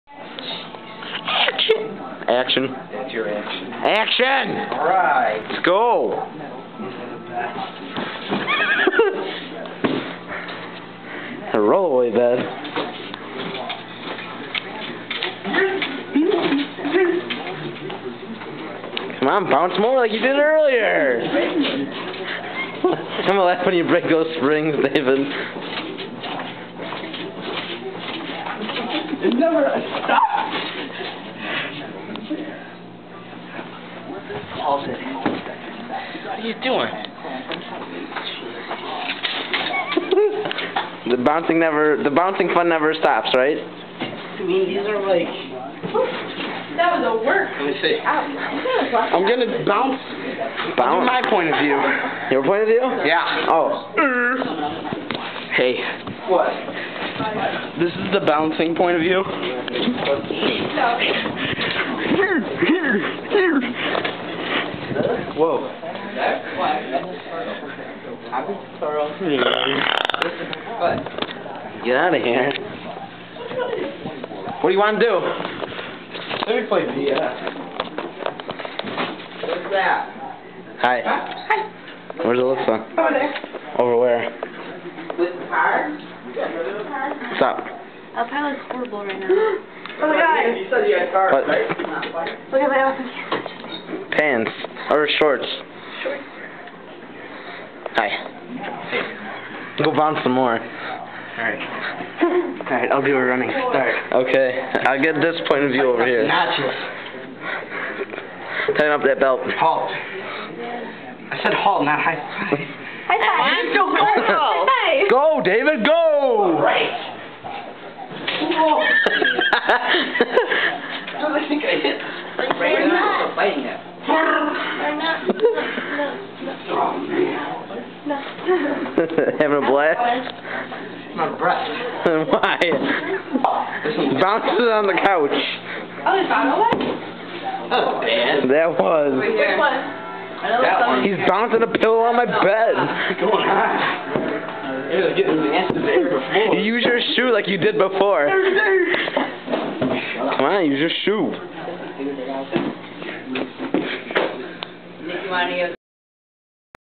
David bouncing on bed
He had a roll away bed for 2 nights and this is what he did every night. Also this was at about 12:15 in the morning